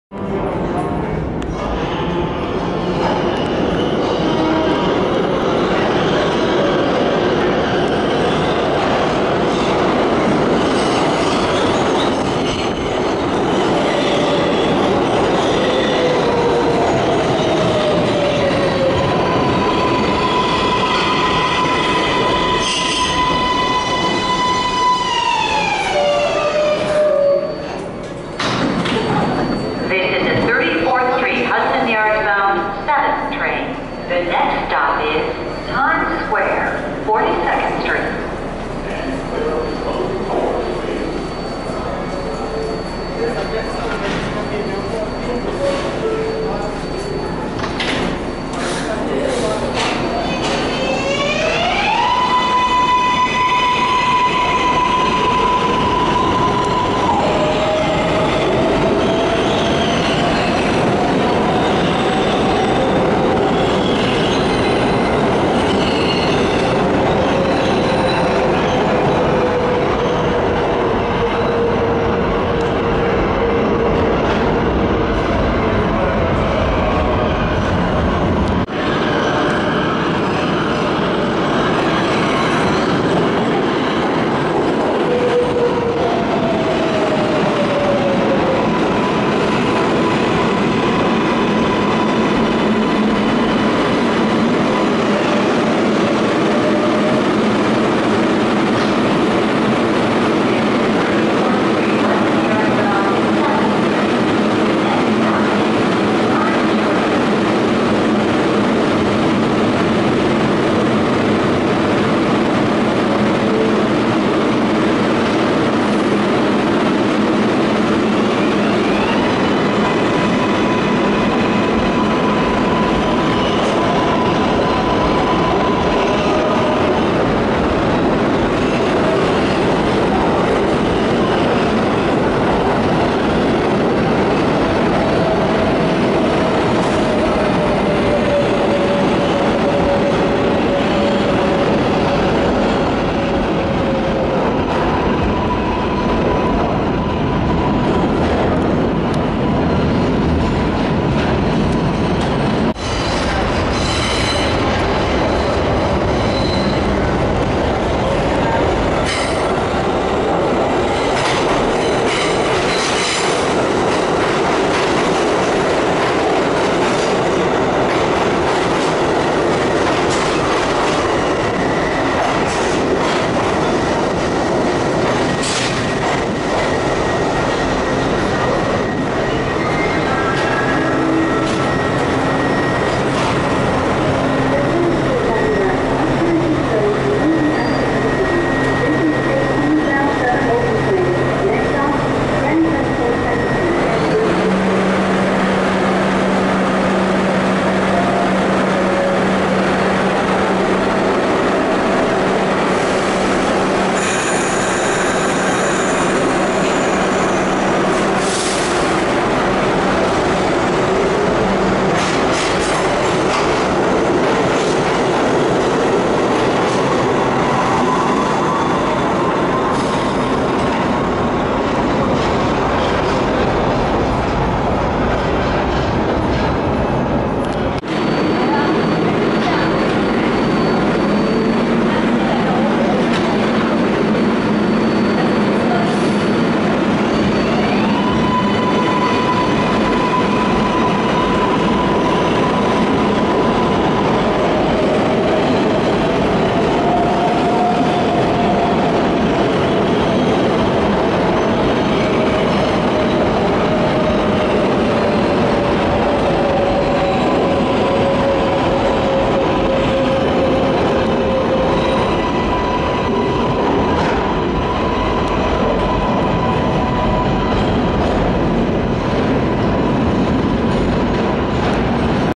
IRT Flushing Line: R188 & R62A 7 Train Action At 42nd St-5th Av-Bryant Park
Route: IRT Flushing Line: Main St (Queens) To 34th St Hudson Yards (Manhattan)

Rolling Stock: R188 Kawasaki

Subway Station: Grand Central

Transfer: (B)(D)(F)(M)Trains

Neighborhood: Hudson Yards, Hells Kitchen

1080p Quality Video Watch In HD